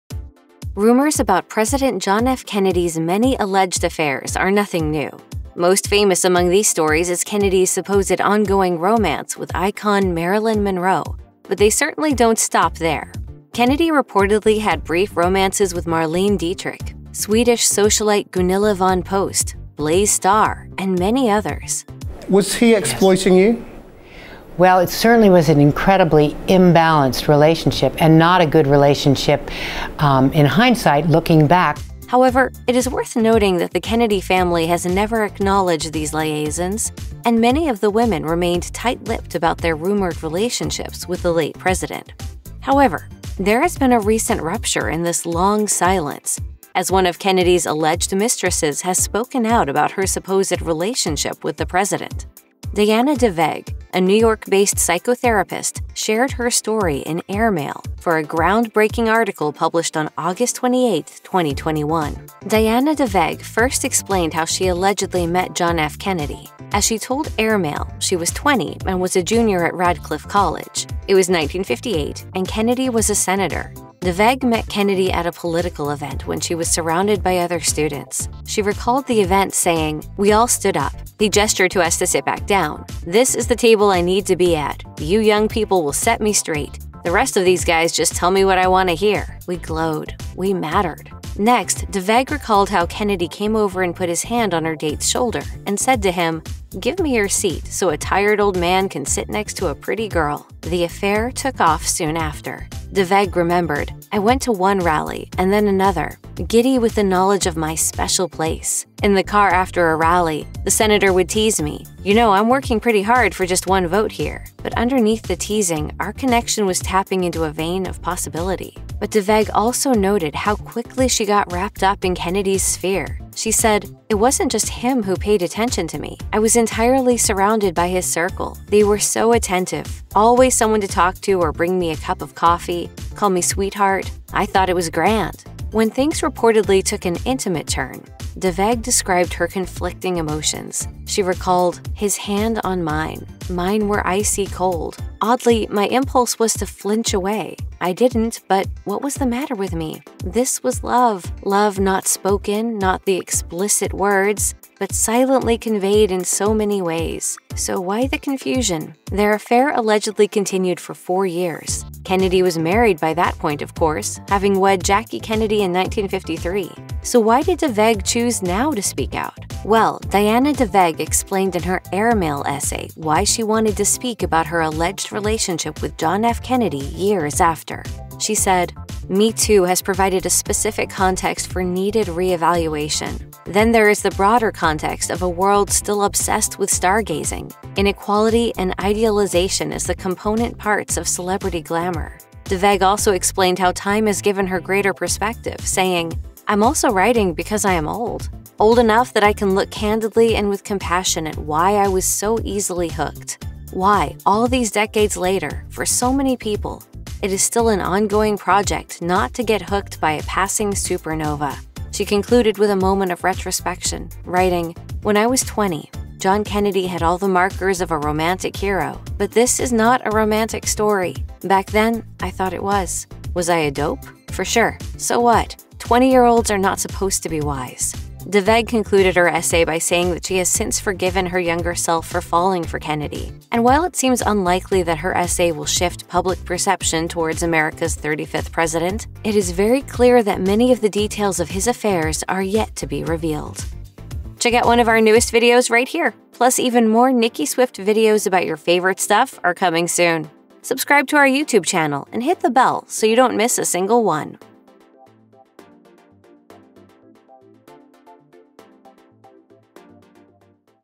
JFK's Alleged Mistress Breaks Her Silence About Their Affair
President John F. Kennedy was known for his many alleged affairs, reportedly having romances with Marilyn Monroe, Marlene Dietrich, Swedish socialite Gunilla Von Post, Blaze Starr, and others. The Kennedy family has never publicly acknowledged these liaisons, and many of the alleged mistresses remain tight-lipped about their supposed relationships with Kennedy. But in August 2021, JFK's alleged mistress Diana de Vegh broke her silence about their affair, which began in 1958, according to de Vegh, when she was a junior at Radcliffe College in Cambridge, Massachusetts. Here's why she came forward, and what she has to say about her time with JFK.